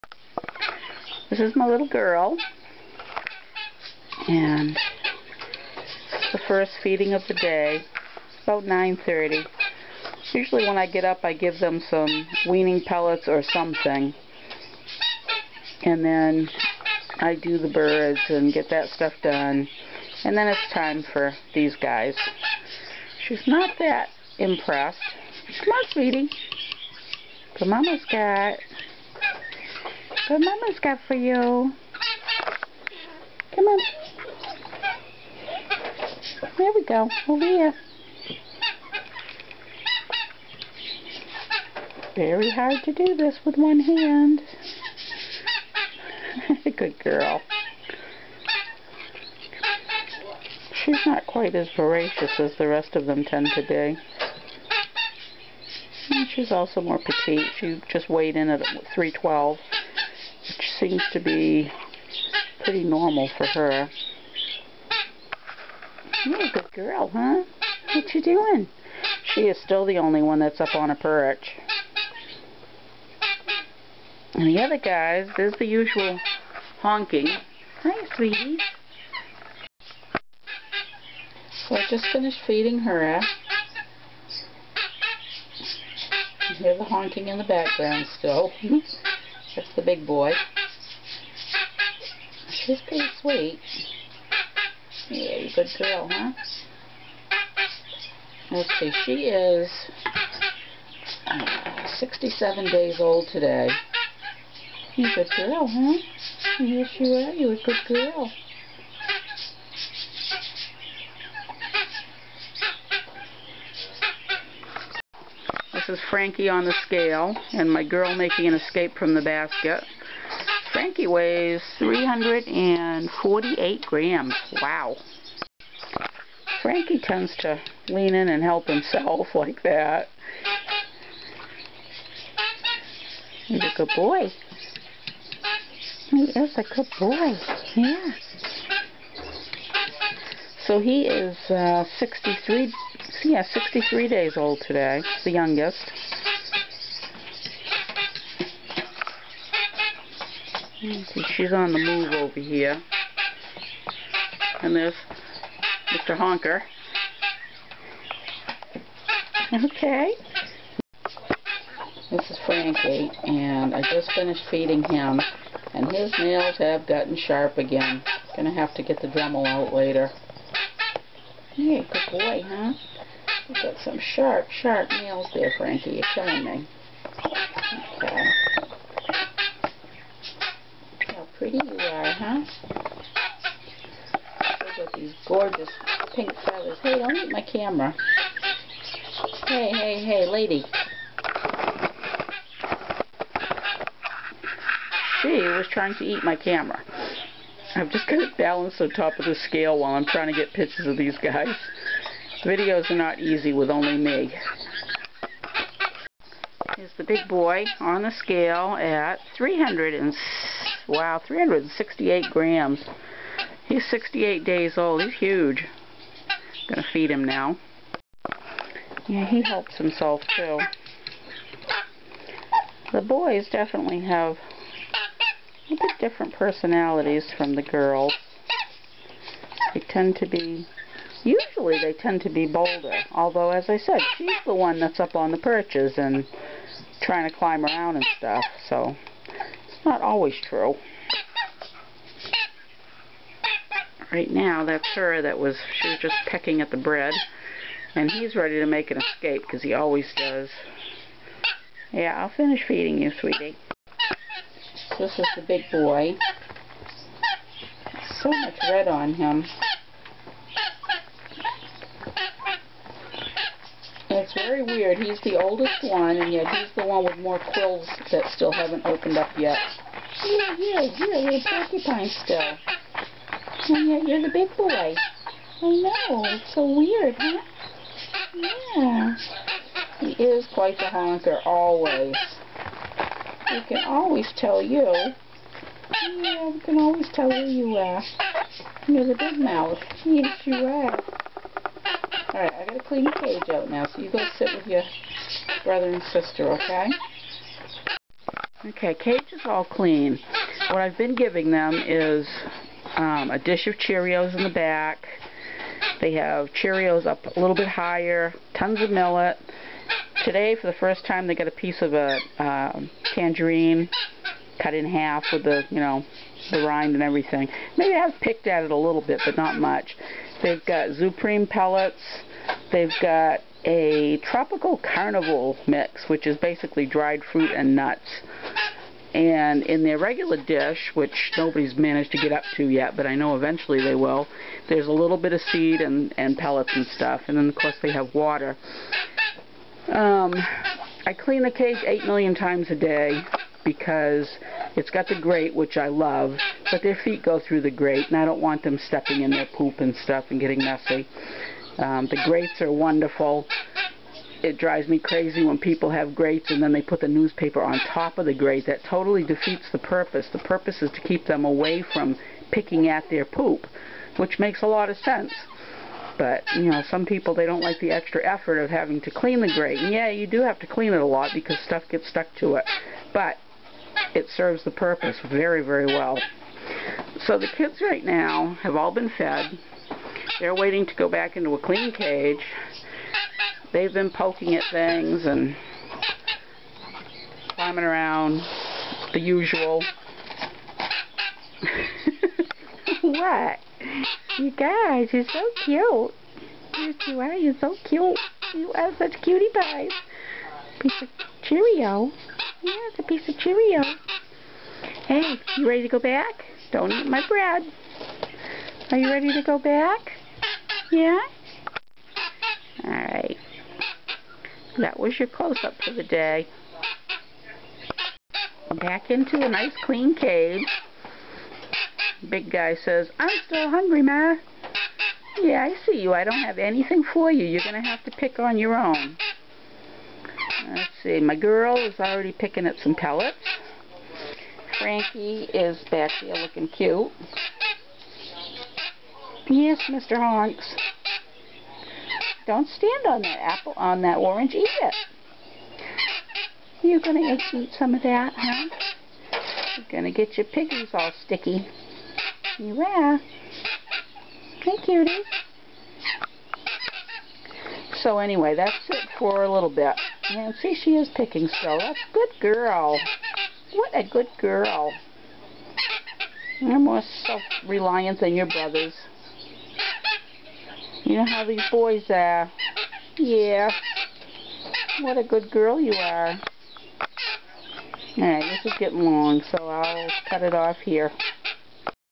01-20017: Timneh Kids' Morning Feeding
A longer video than usual, taken during and after their morning feeding.  Timneh kids are 63, 67, and 68 days old.